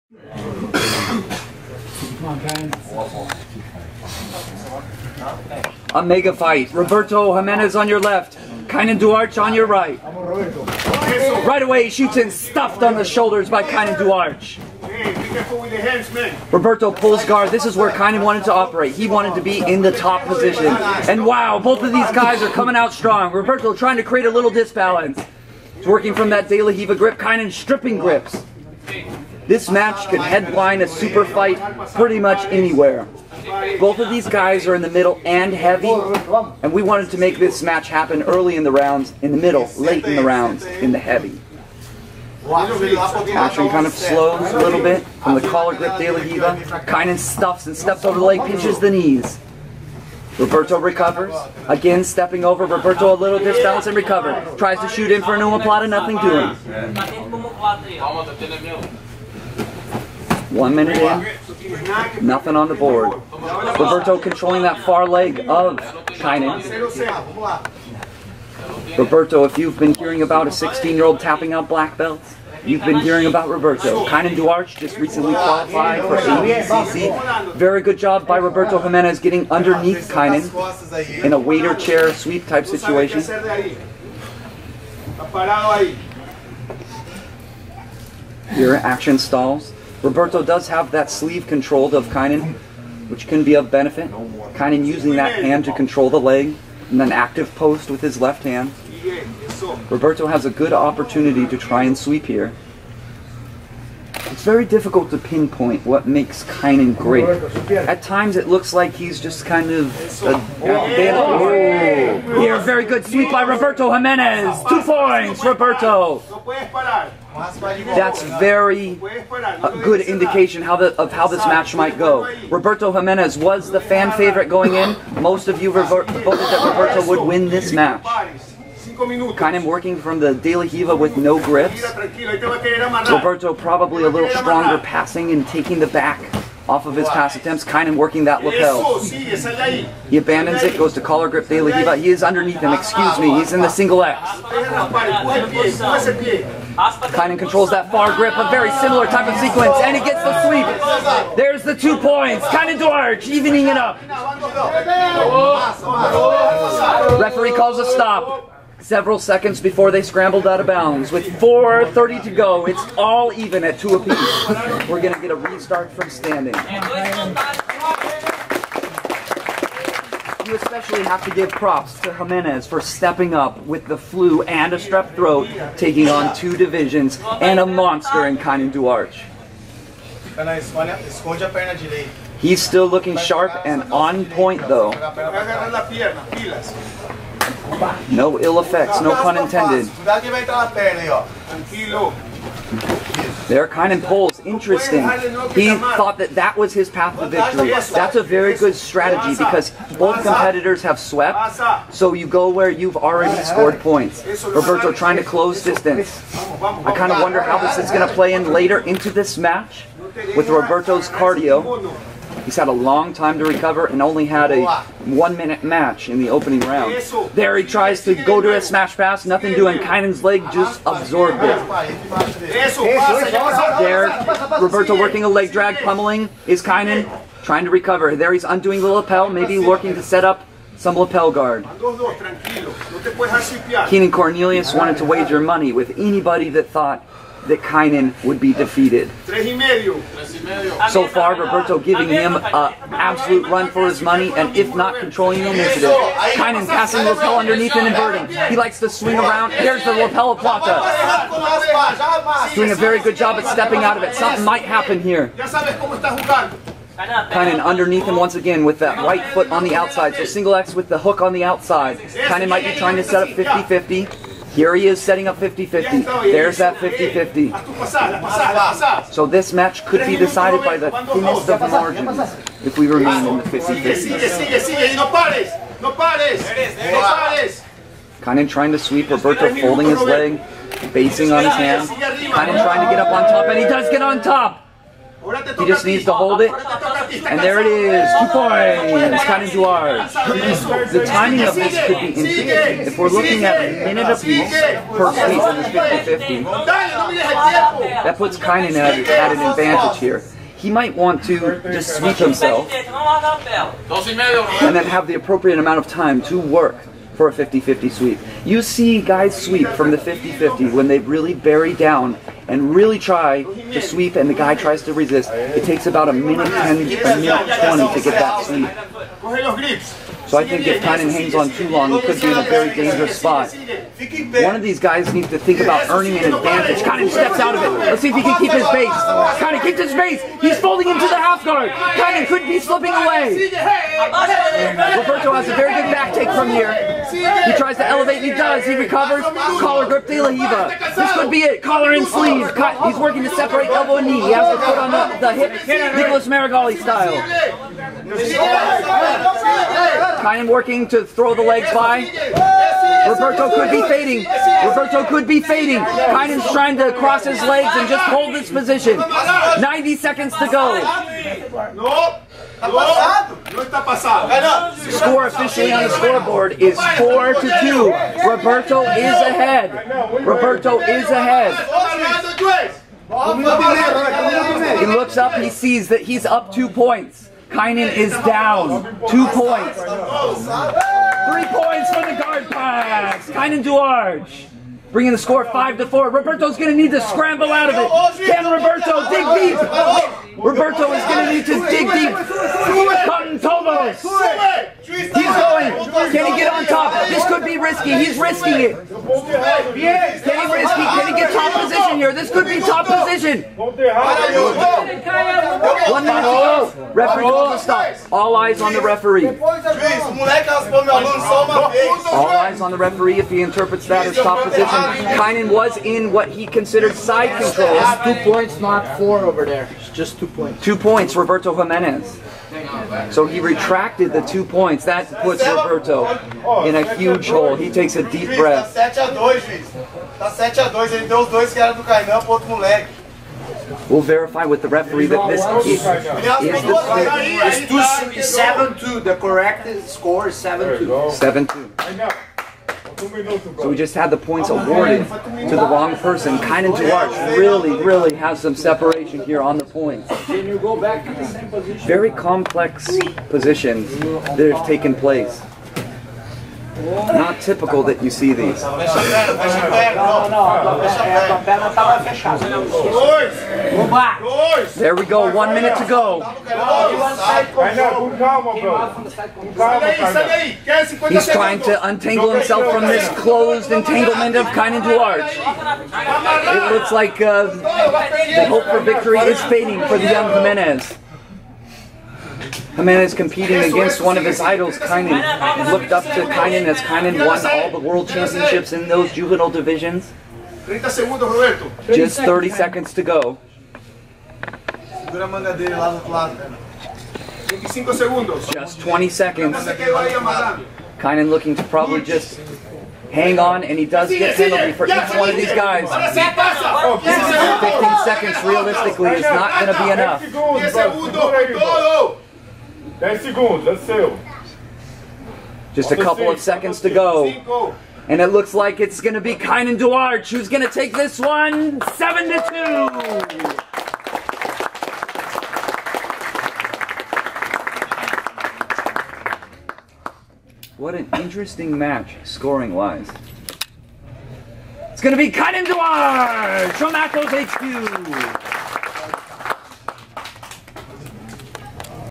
Kaynan Duarte Vs Roberto Jimenez | Jiujitsu.net Open
Battle between the young guns at Jiujitsu.net Open
Purple belt Kaynan Duarte Vs Blue belt  Roberto Jimenez